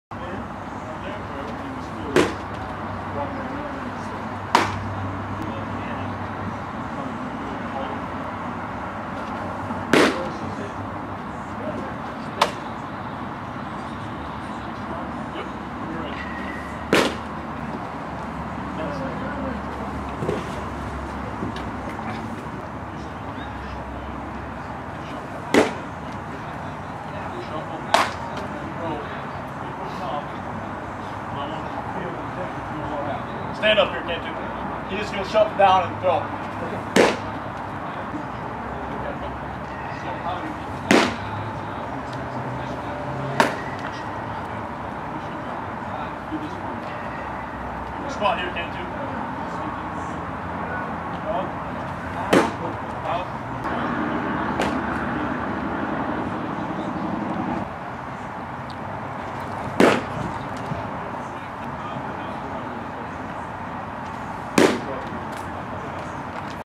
RHP Preston Witt Episcopal High School Class of 2019
Witt putting in work to prep for the 2017 Spring Season